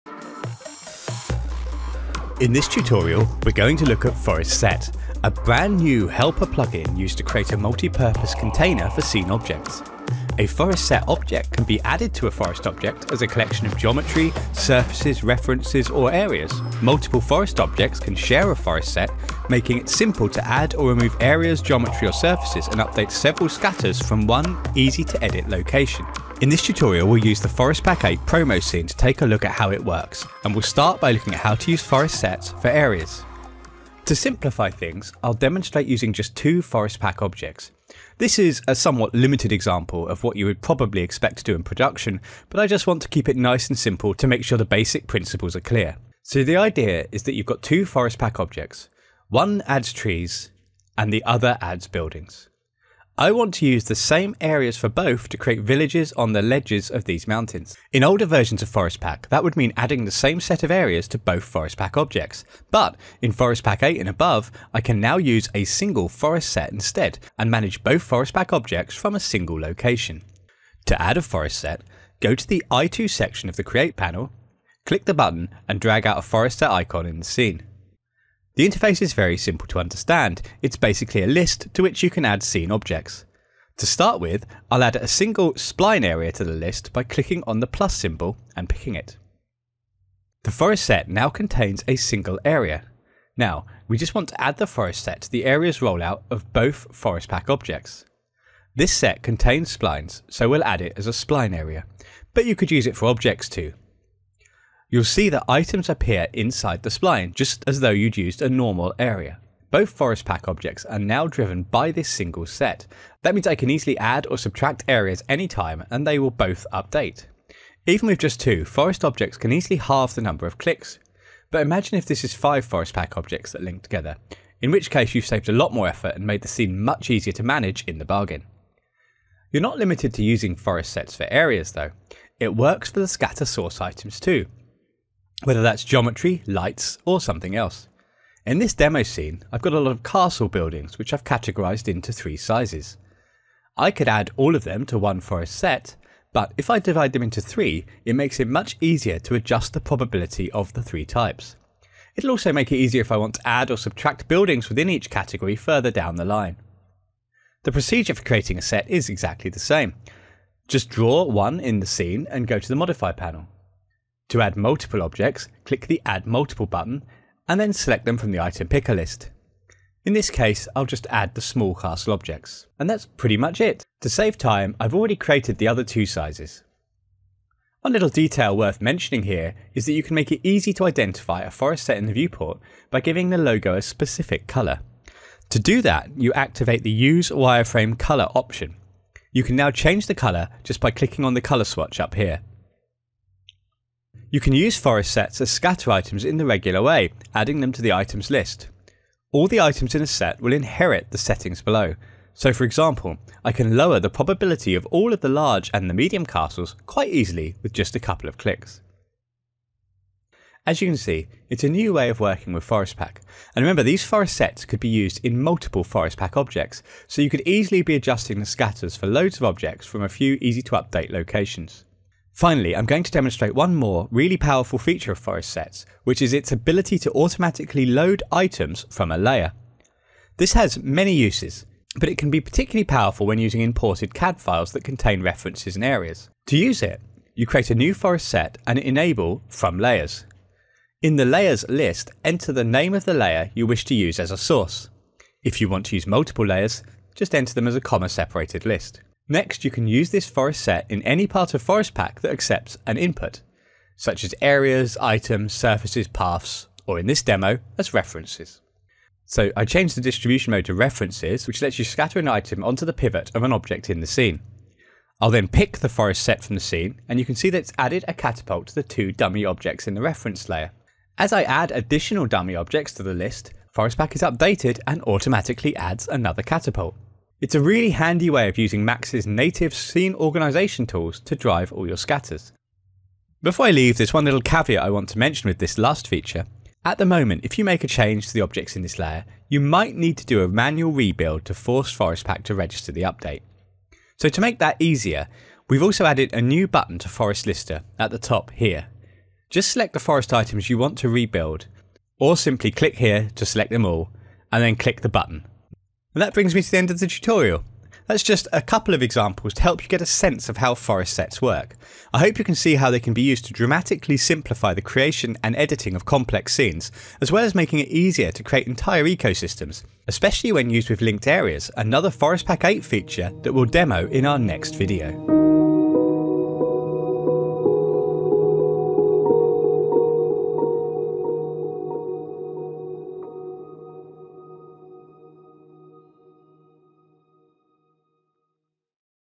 Forest Pack 8 | Introducing Forest Sets
Forest Set is a brand-new helper plugin used to create a multi-purpose container for scene objects. A Forest Set object can be added to a Forest object as a collection of geometry, surfaces, references, or areas. Multiple Forest Objects can share a Forest Set, making it simple to add or remove areas, geometry, or surfaces and update several scatters from one easy-to-edit location. Even better,  the contents of a layer can be dynamically linked to a Forest Set, making it easy to update scatters using familiar scene organisation tools.

++ Music used ++